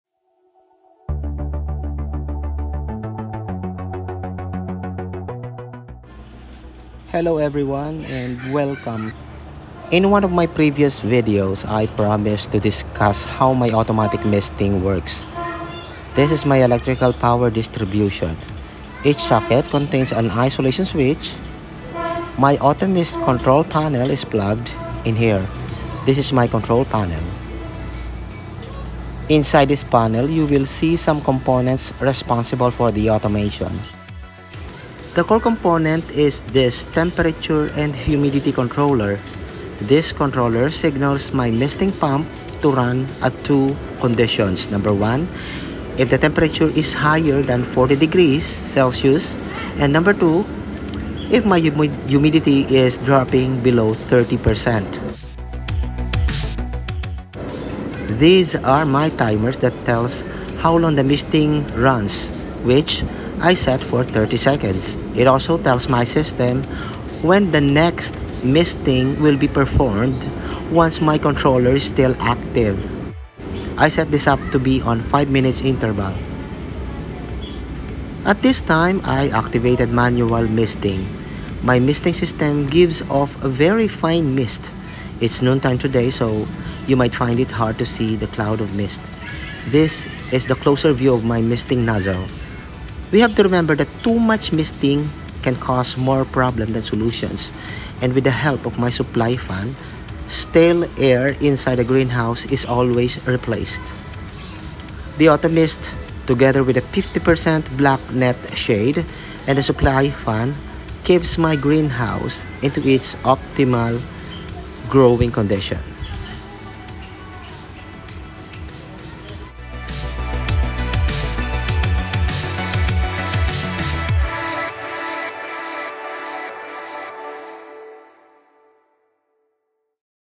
Gab’s Haven - Hydroponics Greenhouse Auto-Mist System
Why Hydroponics? (Part 4)
Ep 6: August 20, 2022

This time, let me iterate all the ones I previously discussed. Advantages:
- Lesser space needed
- Less water
- Greater yield and shorter growing stage

On top of them, other advantages of this soilless farming:
- Less farm waste
- Almost weather proof
- Pest free, minimized pollution effect, outdoor contamination free
- Sustainable

Do you know that NASA has been studying hydroponics as a way to grow food in Mars? :)